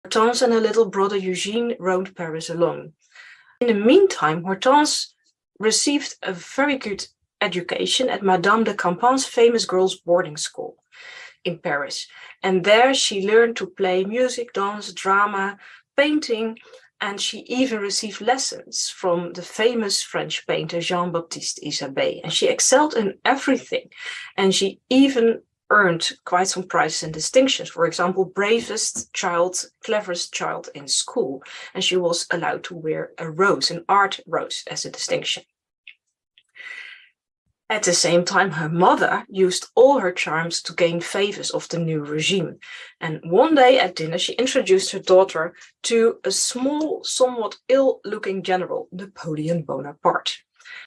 Uncovering Hortenses Incredible Secret The Surprising Connection to Napoleon
We are experimenting with Opus Clip to generate some highlights from some of our live stream events.

This is Beatrice de Graaf speaking on Hortense de Beauharnais